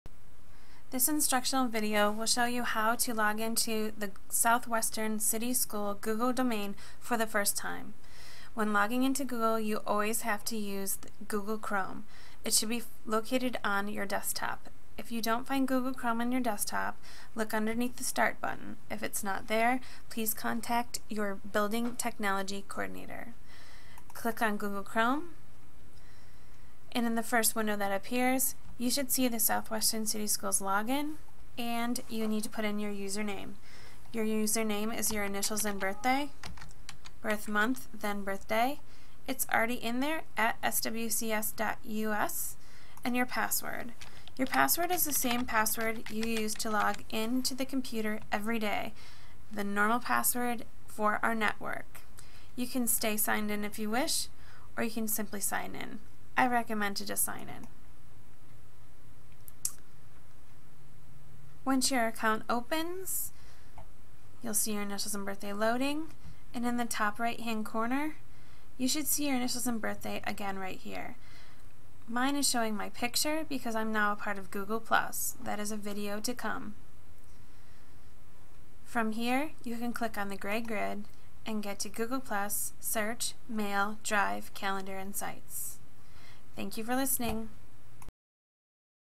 Logging int Our Google Domain-SWCSD
Learning to log into the South-Western City School Domain